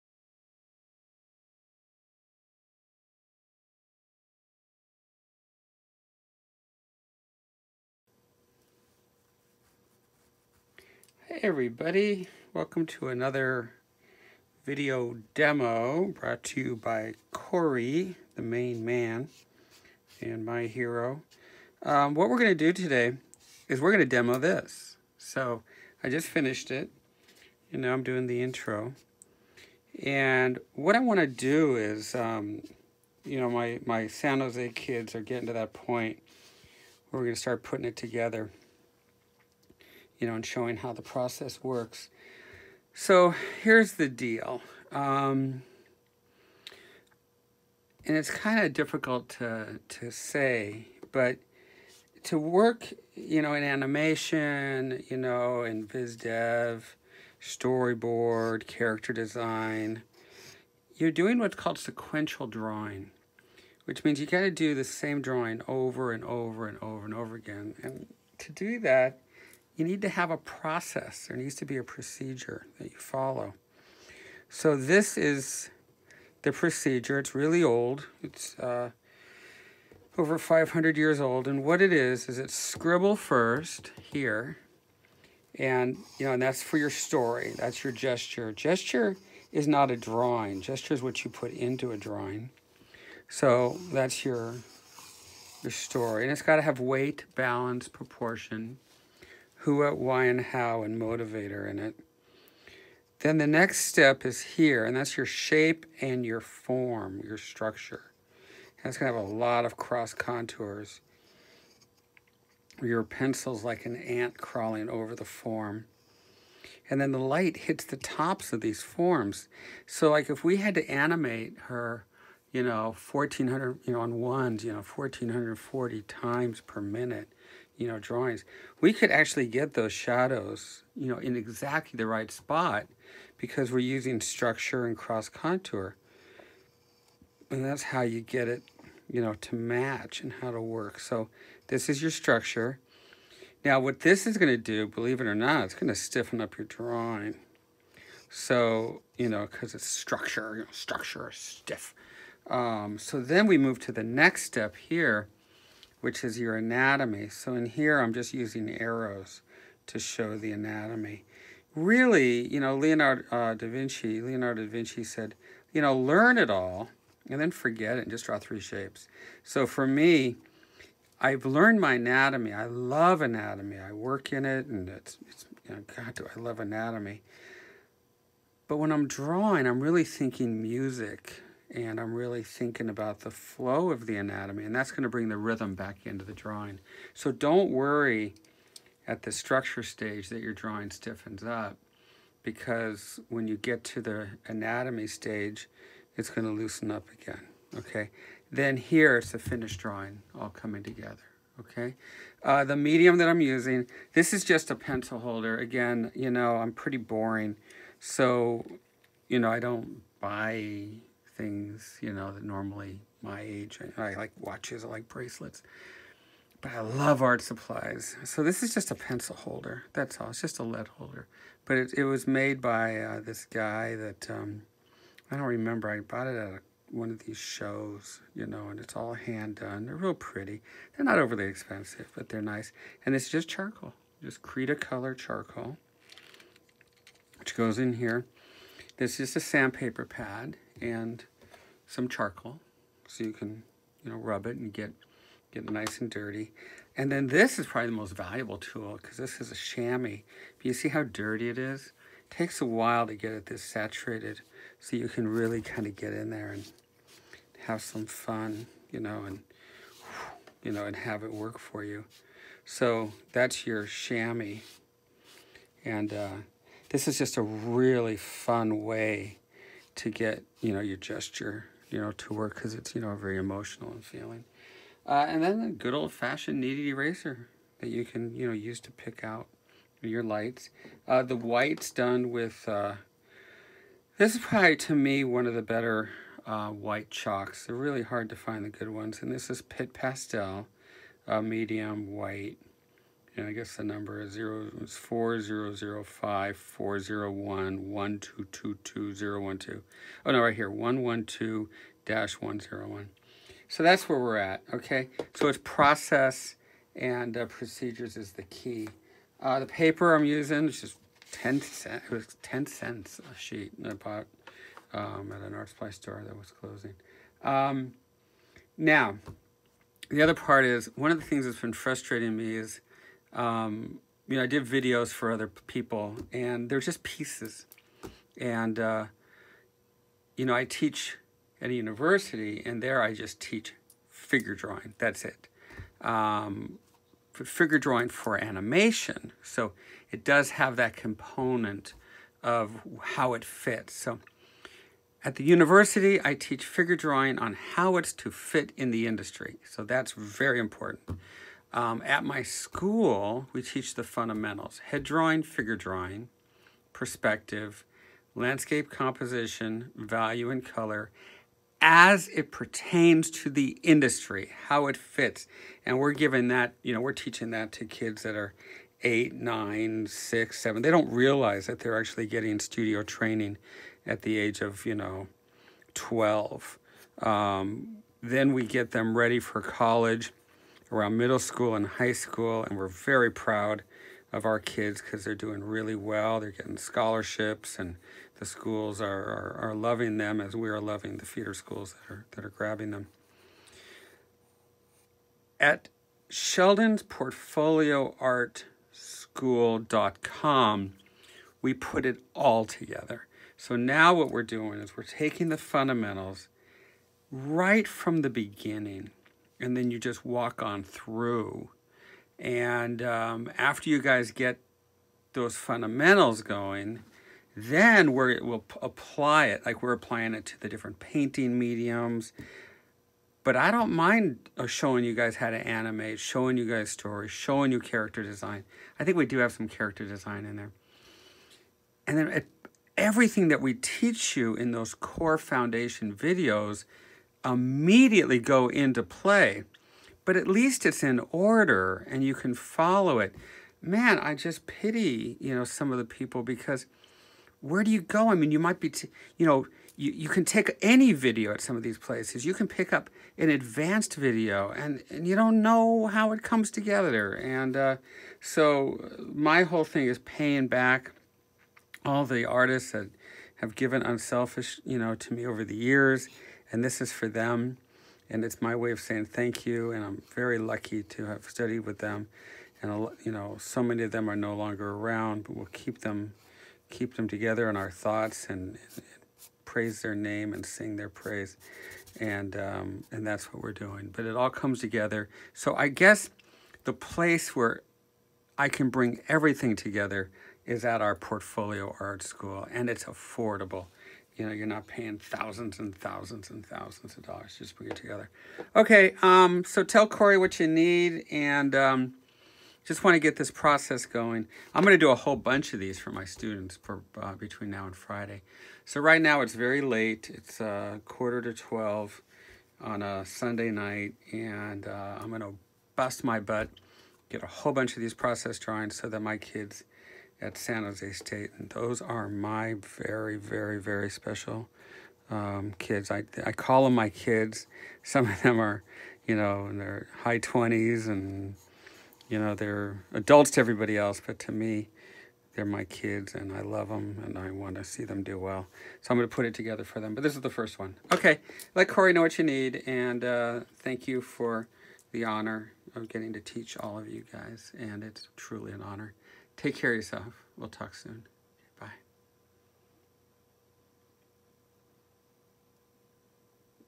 Learning How To Draw The Process to Become a Professional Artist - Sketching With Sheldon #26
Learn Art with Sheldon Borenstein

Sheldon explains the learning process needed for the basics of drawing for Animation, Illustration, Story-boarding.

G.C.A.T
1)Scribble - Gesture
2)Shape/Structure - Construction
3)Rhythms/Flow - Anatomy
4)combine and add Technique

Powder Charcoal
Charcoal lead and holder
Sand paper
toned paper
Pitt Pastel